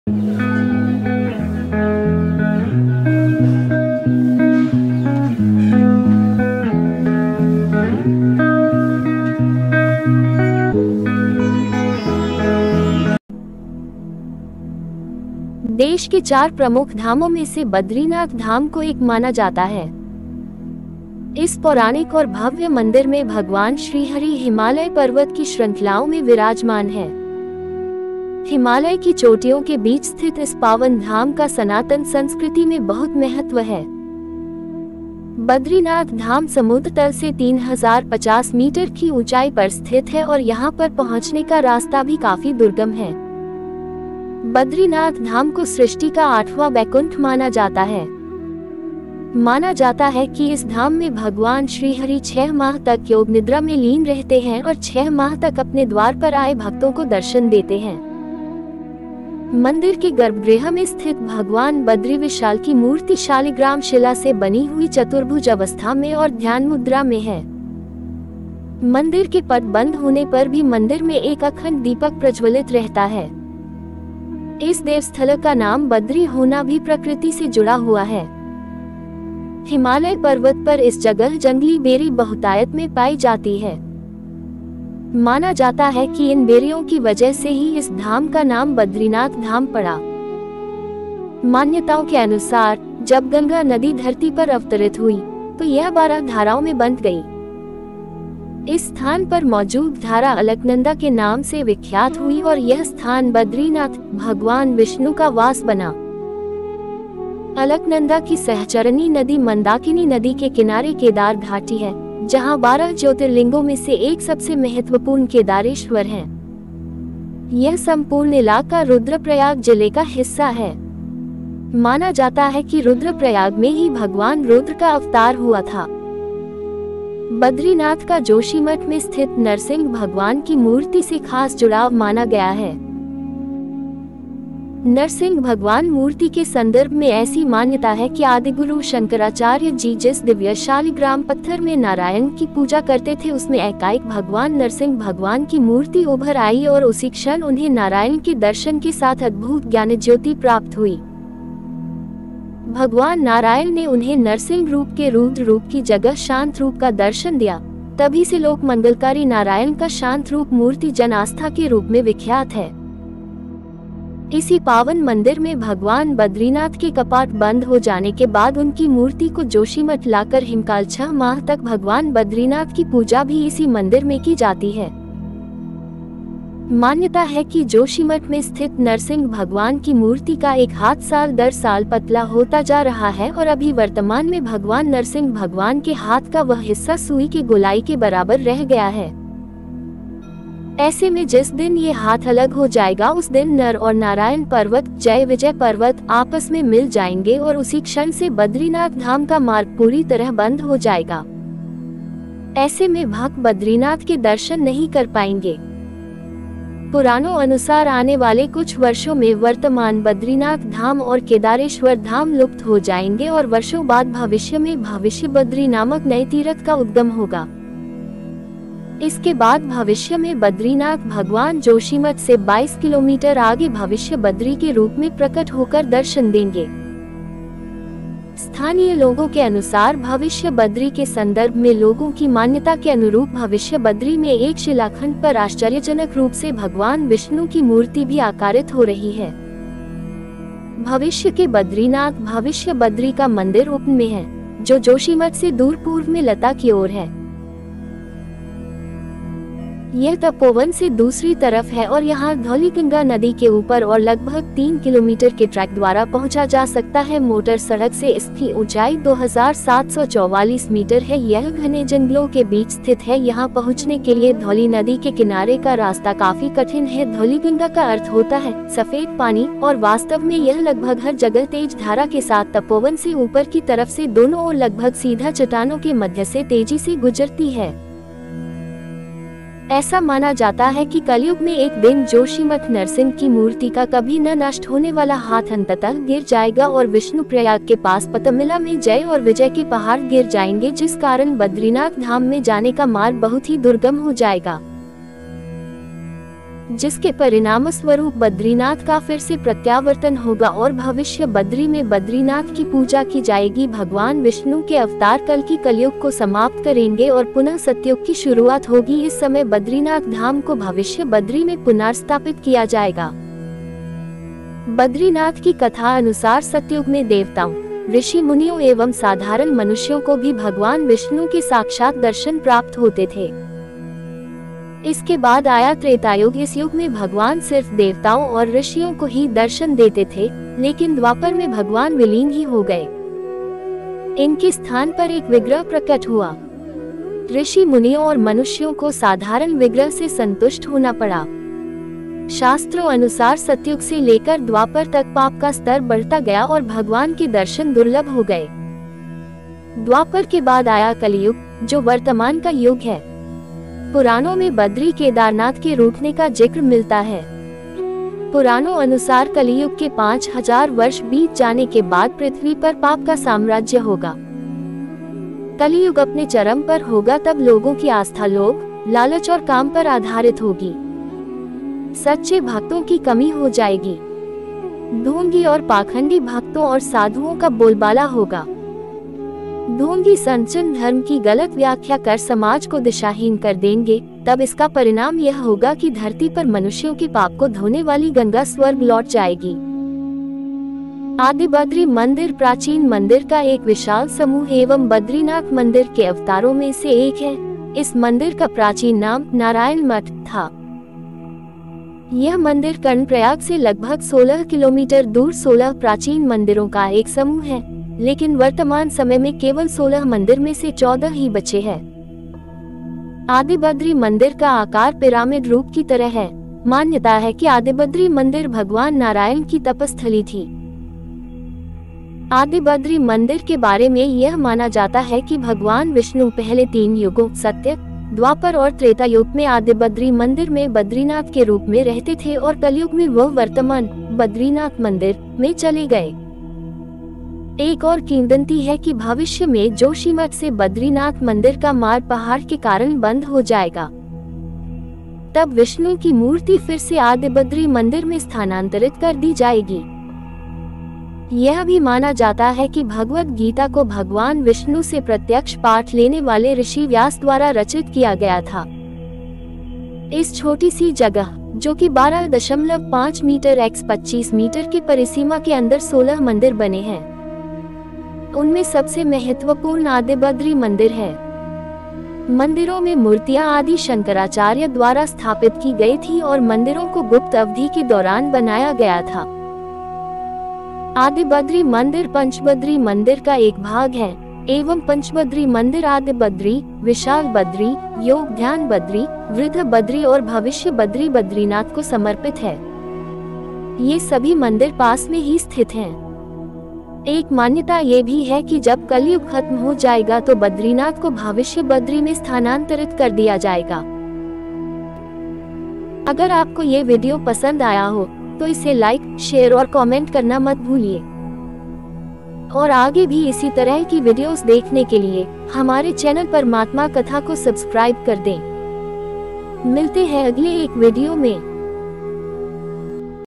कलयुग के अंत तक लुप्त होजायेगा बद्रीनाथ मंदिर। secrets of badrinath temple।
माना जाता है। इस पौराणिक और भव्य मंदिर में भगवान श्रीहरी हिमालय पर्वत की श्रंखलाओं में विराजमान है। हिमालय की चोटियों के बीच स्थित इस पावन धाम का सनातन संस्कृति में बहुत महत्व है।बद्रीनाथ धाम समुद्र तल से 3,050 मीटर की ऊंचाई पर स्थित है और यहां पर पहुंचने का रास्ता भी काफी दुर्गम है।